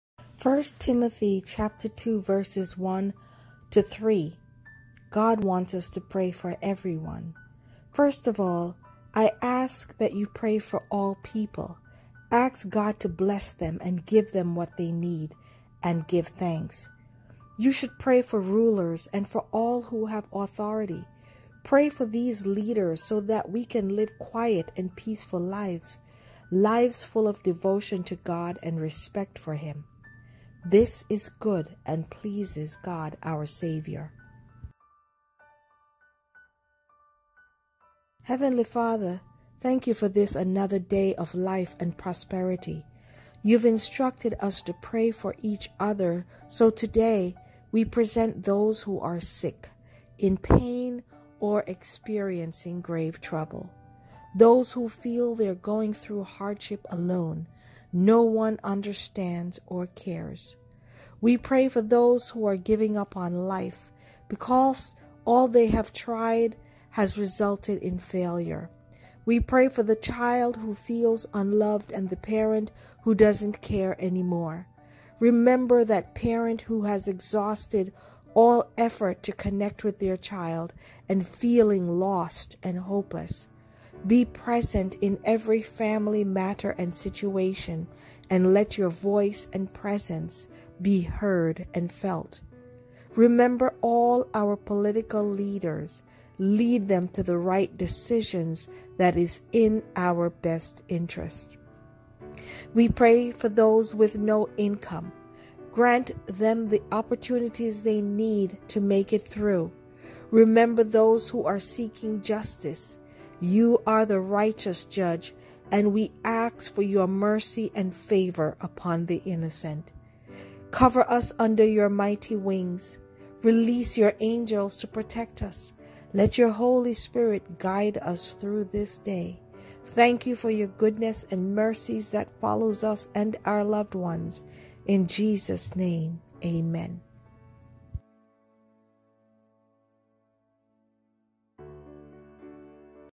Prayer List February 15, 2021
Heavenly Father,
Thank you for this another day of life and prosperity.
You’ve instructed us to pray for each other, so today we present those  who are sick, in pain or experiencing grave trouble.  Those who feel they are going through hardship alone, no one understands or cares.  We pray for those are giving up on life, because all they have tried has resulted in failure.
We pray for the child who feels unloved and the parent who doesn’t care anymore.  Remember that parent who has exhausted all effort to connect with their child and feeling lost and hopeless.  Be present in every family matter and situation,  let your voice and presence be heard and felt.
Remember all our political leaders, lead them to the right decisions that is in our best interest.
We pray for those with no income, grant them the opportunities they need to make it through.  Remember those who are seeking justice, you are the righteous judge, and we ask for your mercy and favor upon the innocent.
Cover us under your mighty wings, release your angels to protect us, let your Holy Spirit guide us through this day.
Thank you for goodness and mercies that follows us and our loved ones.